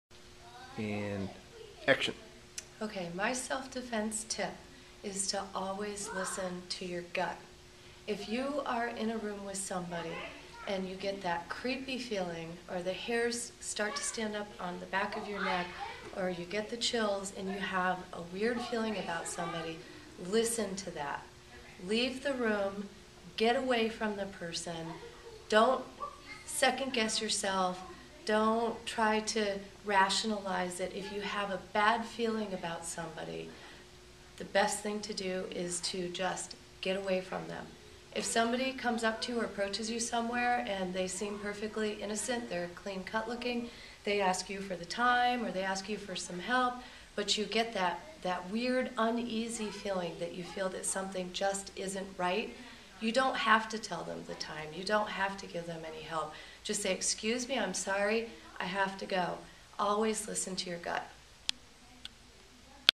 Always Listen To Your Gut
Sensei Heidi can be seen training around Integrity Martial Arts with her kids.  She is training for black belt now and is the survivor of some real life self defense situations.